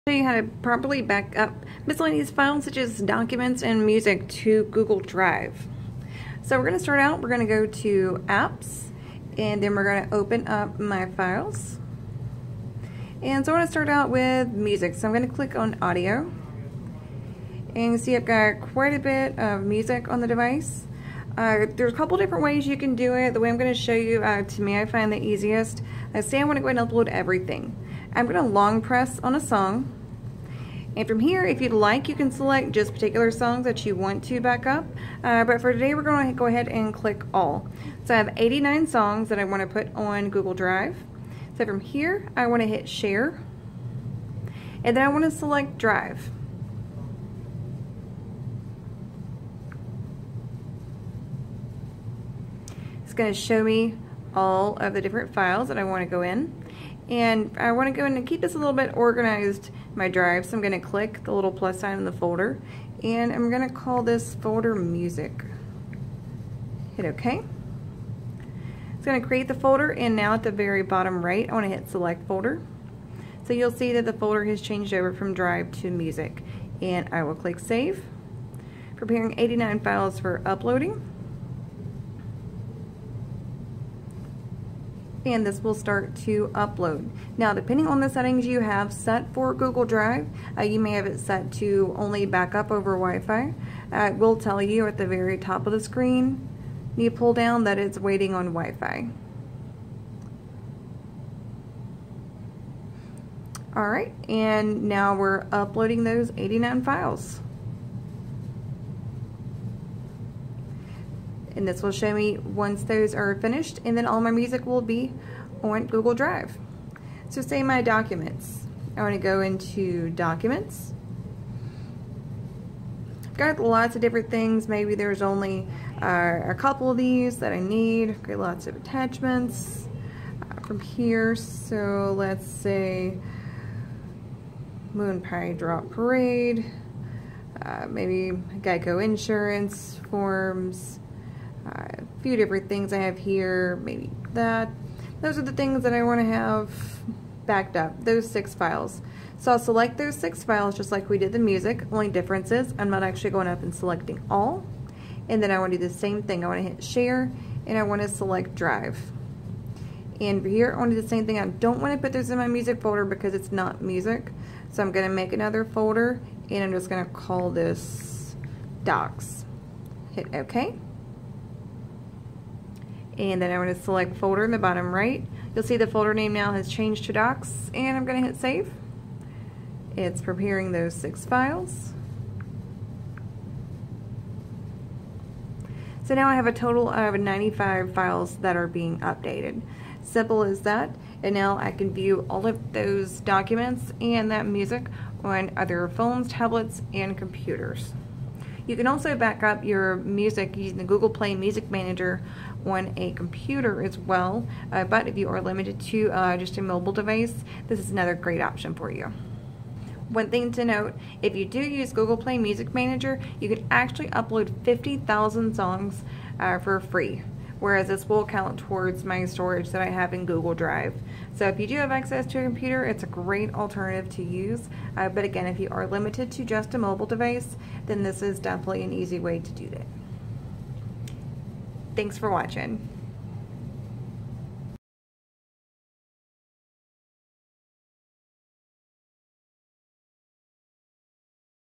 How to Back Up Documents and Music to Google Drive
This video will walk you through uploading your documents and music to Google Drive.